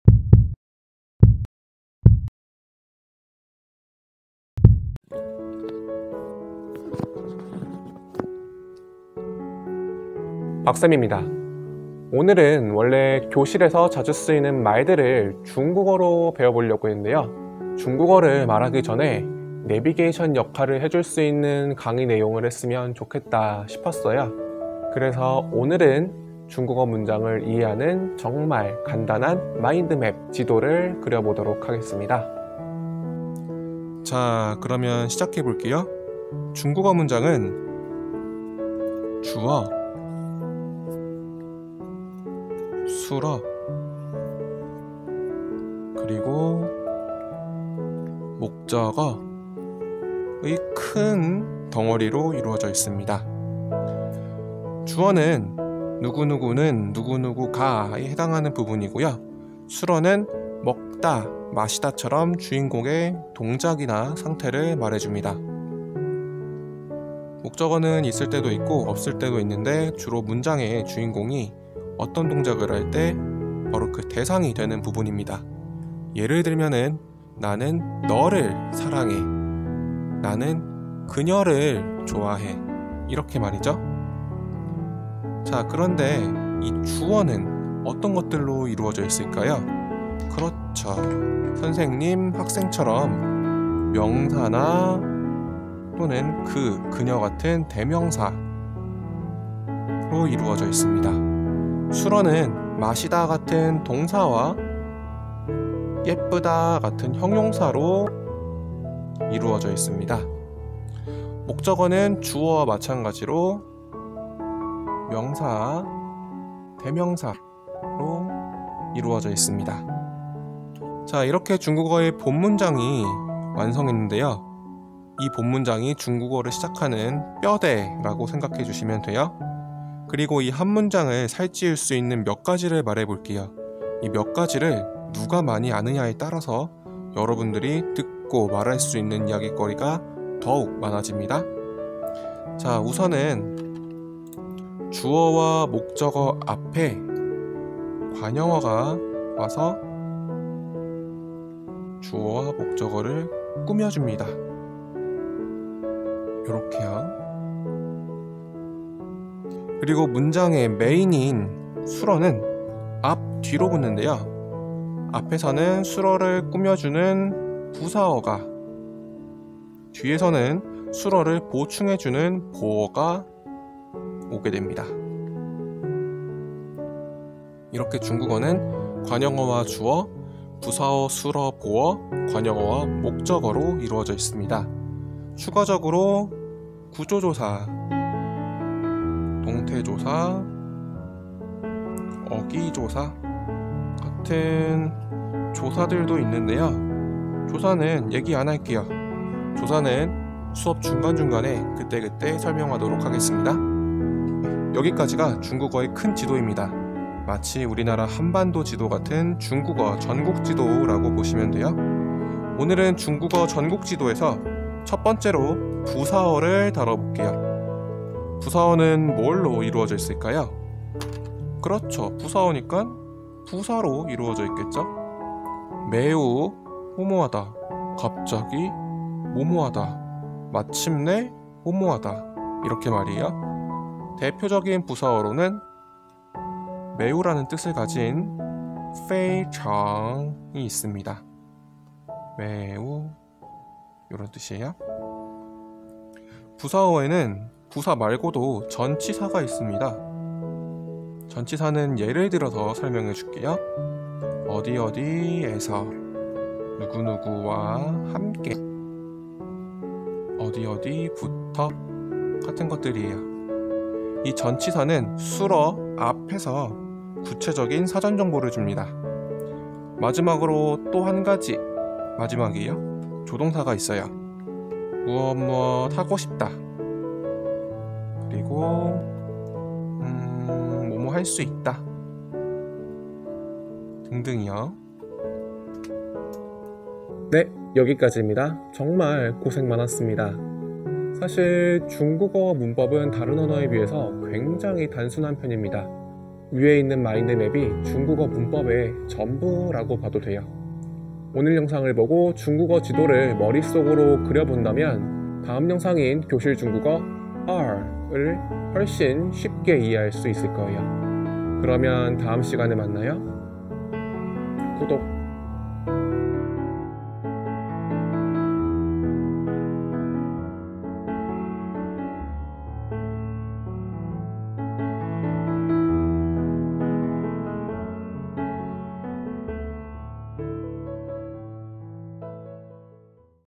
[예일디자인고등학교/온라인클래스/중국어1][5주차 / 5월 14일~] 중국어 문장구조(1) (p.8 에 필기!)
중국어의 문장구조를 이야기해 보았어요~!

5분만 집중해봐요 ^^!!